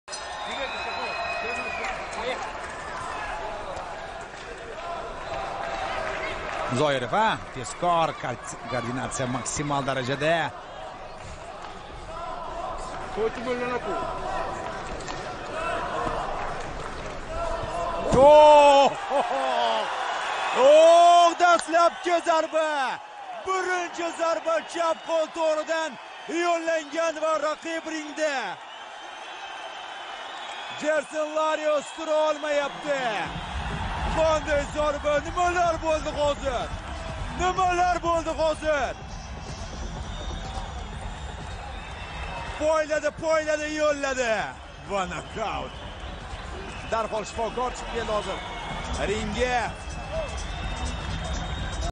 Shahobiddin Zoirov Toshkentda o‘tgan boks oqshomida raqibini 1-raunddayoq nokautga uchratdi
Toshkent shahar IIBB xodimi Shahobiddin Zoirov Toshkentda o‘tgan boks oqshomida raqibini 1-raunddayoq nokautga uchratdi

Jang 1 daqiqa ham davom etmadi.